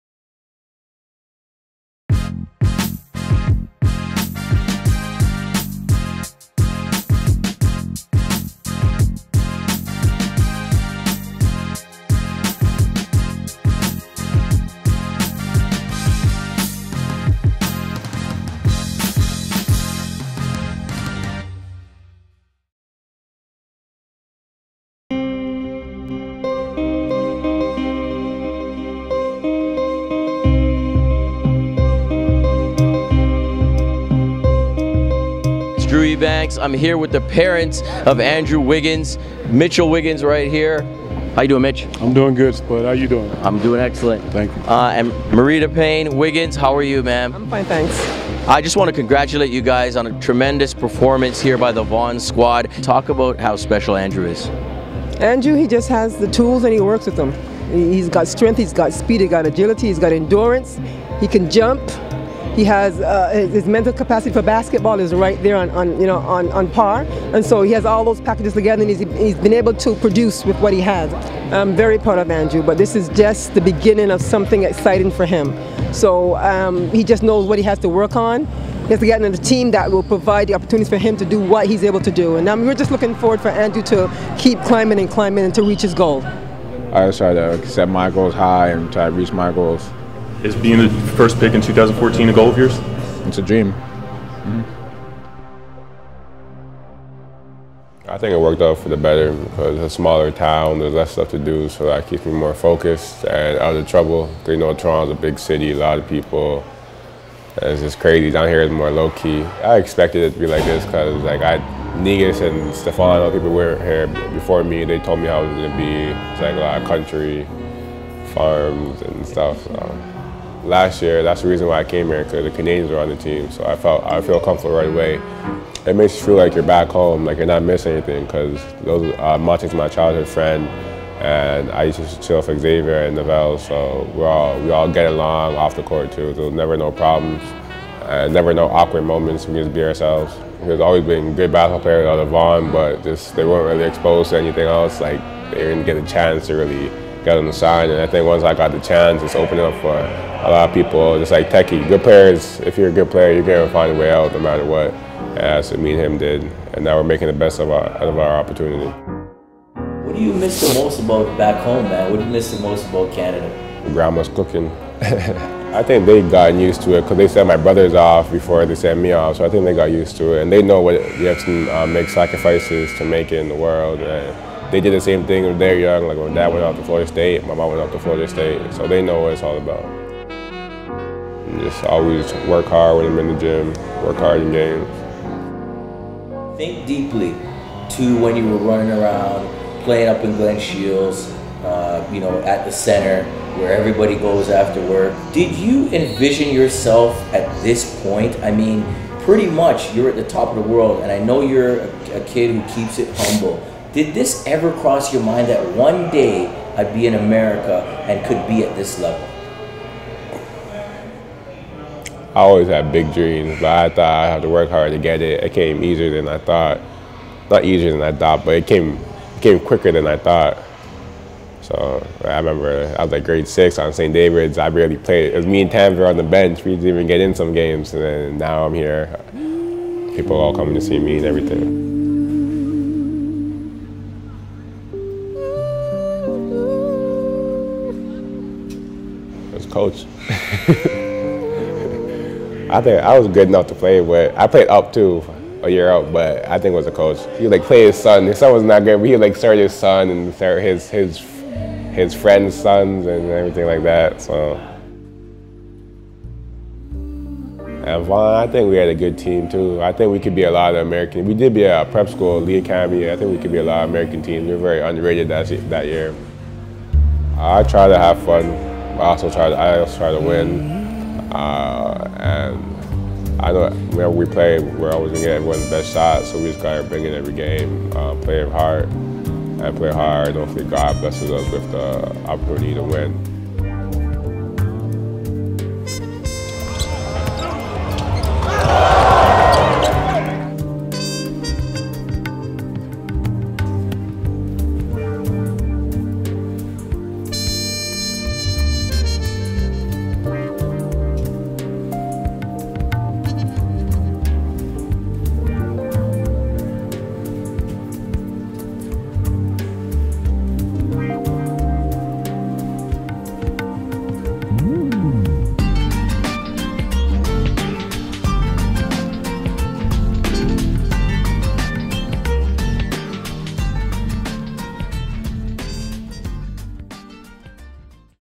Andrew Wiggins - Never Before Seen Footage
Never before seen interview footage of Andrew Wiggins from the cutting room floor. Bonus interview footage and broll of Andrew's Mom (Marita Payne-Wiggins and his Dad (Mitchell Wiggins) from 2011. Andrew Wiggins will soon be starting his freshman season for the Kansas Jayhawks, a pivotal year in his early career.

Produced by Drew Ebanks & Jay Irving
Filmed & Edited by Jay Irving
Hosted by Drew Ebanks
Time-lapse footage by David Zelikovitz
Additional footage by Drew Ebanks
Graphics by Mark Sylvain